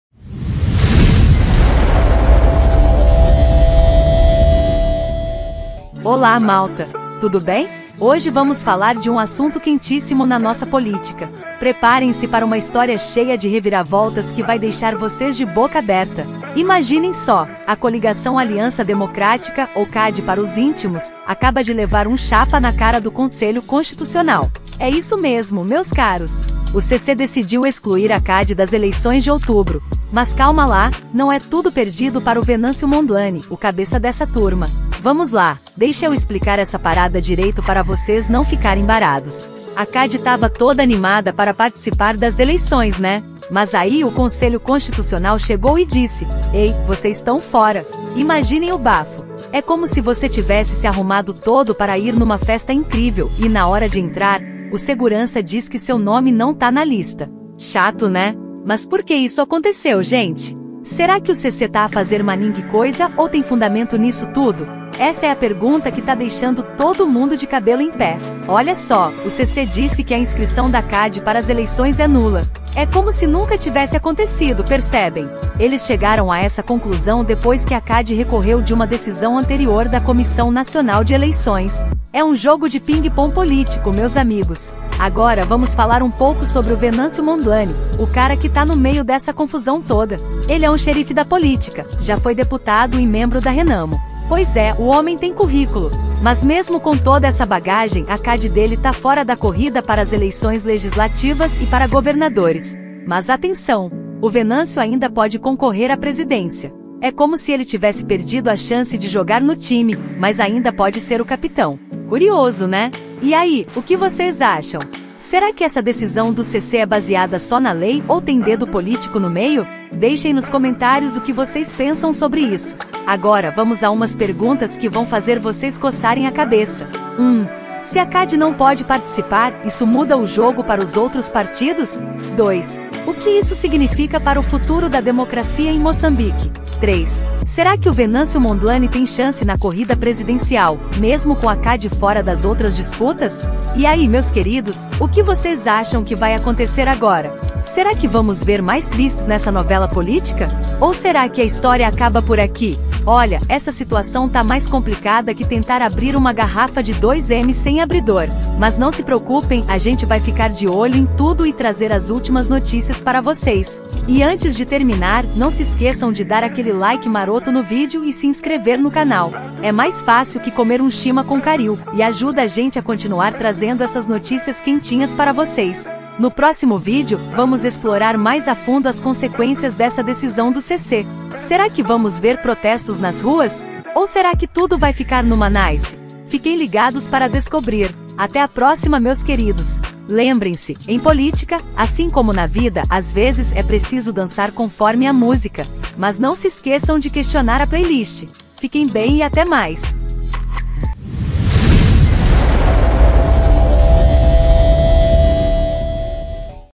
CAD Excluída das Eleições: Decisão Polêmica do CC | Últimas Notícias
🇲🇿 O Conselho Constitucional de Moçambique excluiu a Coligação Aliança Democrática (CAD) das eleições de outubro. Entenda o impacto dessa decisão na política moçambicana e o que isso significa para o futuro do país.

Neste vídeo, analisamos:
✅ A decisão do CC e seus fundamentos
✅ O futuro da candidatura de Venâncio Mondlane
✅ As implicações para a democracia em Moçambique

💬 Deixe sua opinião nos comentários: Você acha que essa decisão é justa?

Palavras-chave SEO:
Eleições Moçambique 2024
CAD excluída eleições
Conselho Constitucional Moçambique
Venâncio Mondlane candidatura
Política Moçambicana atualidades